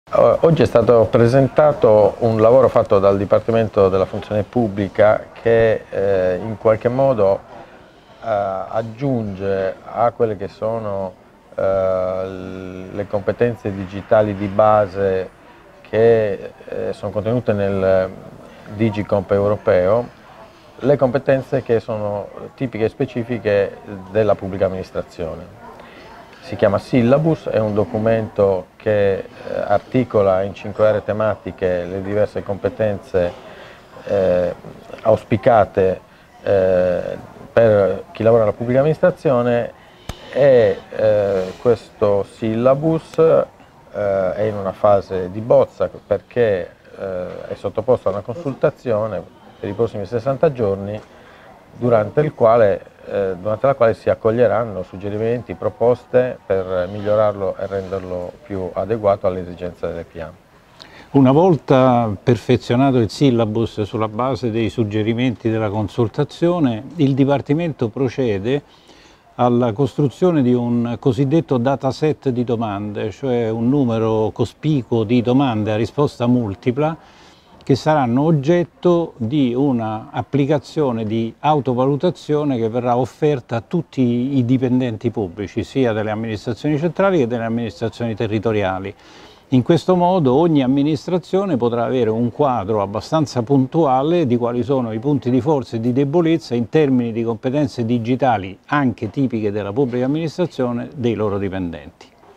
Syllabus per le competenze digitali per la PA presentato a ICity Lab 2018
Salvatore Marras, Responsabile Area Innovazione Digitale - Formez PA, e Franco Patini, Confindustria Digitale, presentano il Syllabus, primo atto del modello di sviluppo delle competenze digitali nella PA promosso dal Dipartimento della Funzione Pubblica.